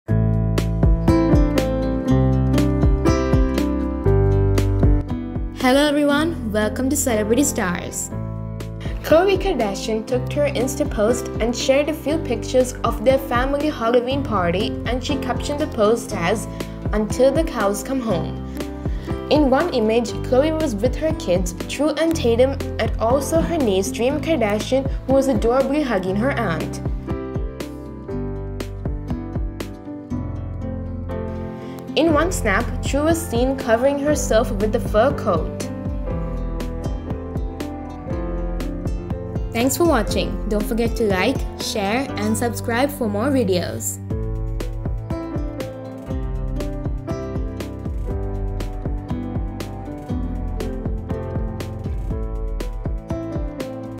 Khloe Kardashian Shared Snaps of Their Family Halloween Party
khloe kardashian took to her insta post and shared few pictures of their family halloween party and she captioned the post as, "until the cows come home".